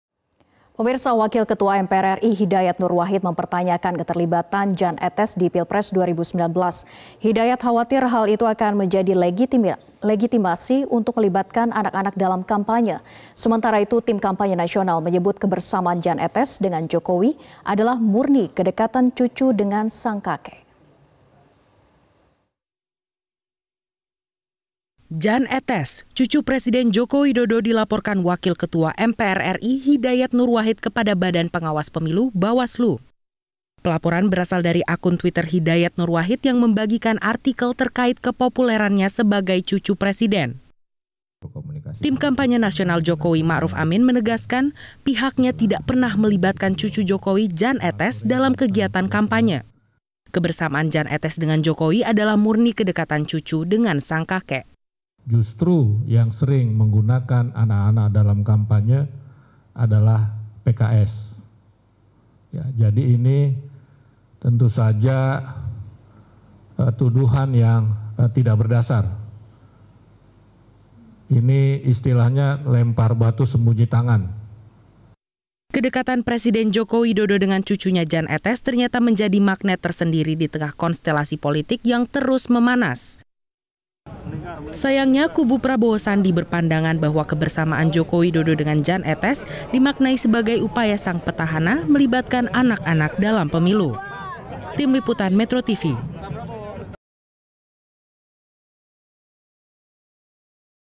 Jan Ethes Disindir soal Kampanye Jokowi
Wakil Ketua MPR RI Hidayat Nur Wahid mempertanyakan keterlibatan Jan Ethes di Pilpres 2019. Hidayat khawatir hal itu akan menjadi legitimasi untuk melibatkan anak-anak dalam kampanye. Sementara itu Tim Kampanye Nasional (TKN) menyebut kebersamaan Jan Ethes dengan Jokowi adalah murni kedekatan cucu dengan sang kakek.